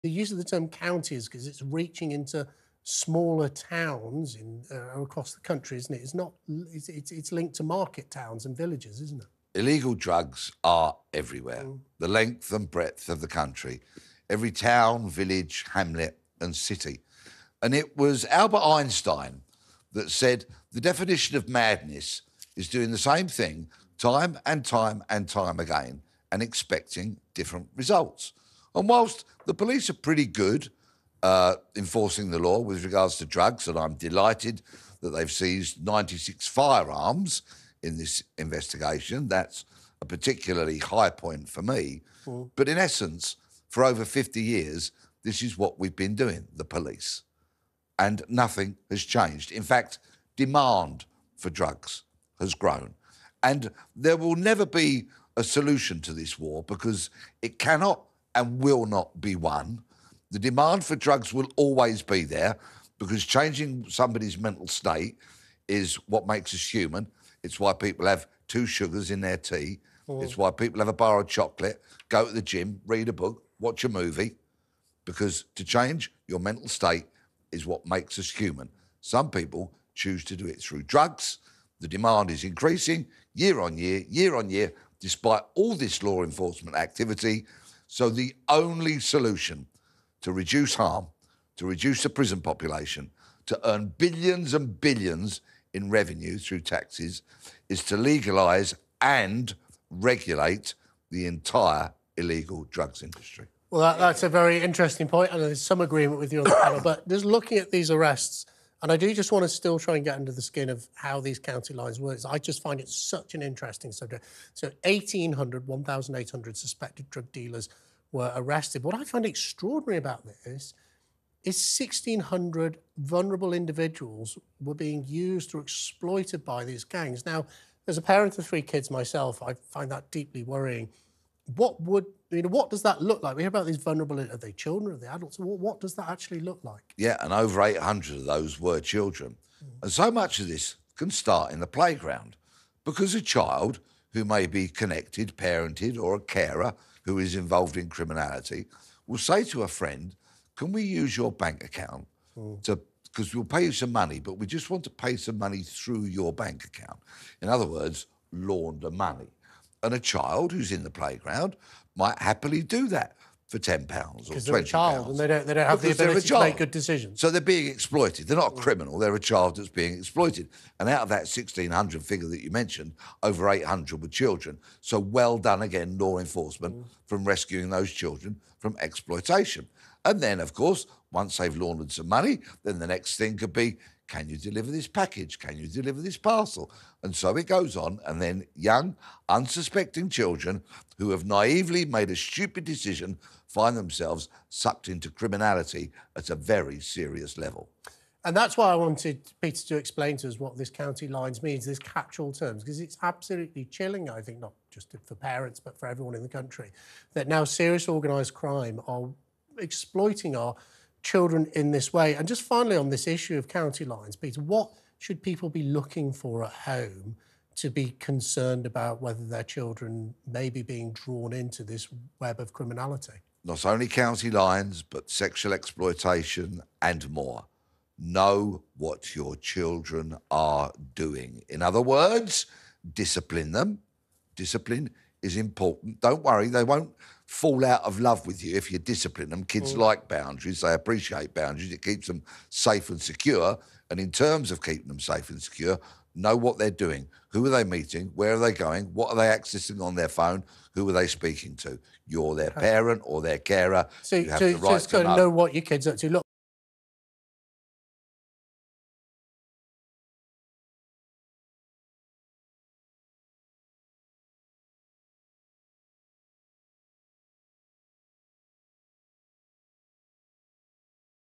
Peter Blekesly Cautions Parents On How To Prevent Children Becoming Involved with Organised Crime
Illegal drugs and weapons have been seized in a police operation targeting county lines gangs in North Yorkshire.

Thirteen people were arrested during a week-long operation between 4-10 March, police said.

The raids saw Class A and Class B drugs confiscated, along with a machete, a crossbow and a throwing knife.

TalkTV's Jake Berry asks former Met Detective Peter Bleksley how parents and carers can keep children safe against organised crime.